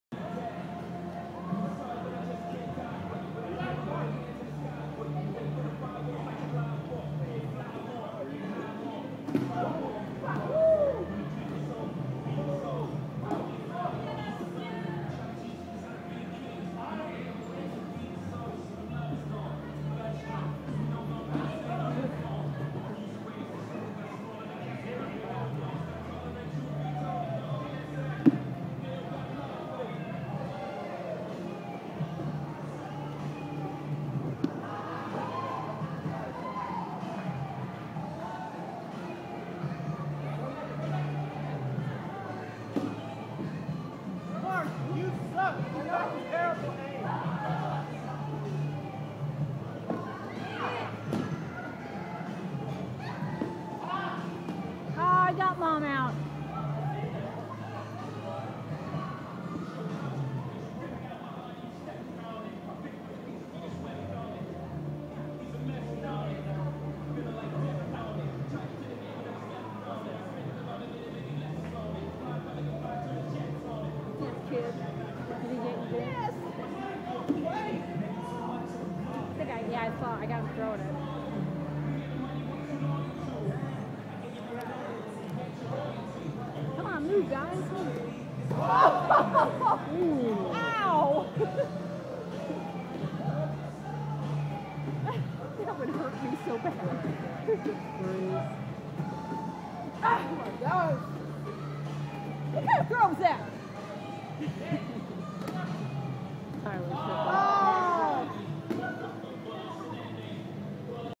Indy SkyZone / April 7th
Dodgeball w/ CJ, Corri, Sam, Tyler Jeanne & Kimberley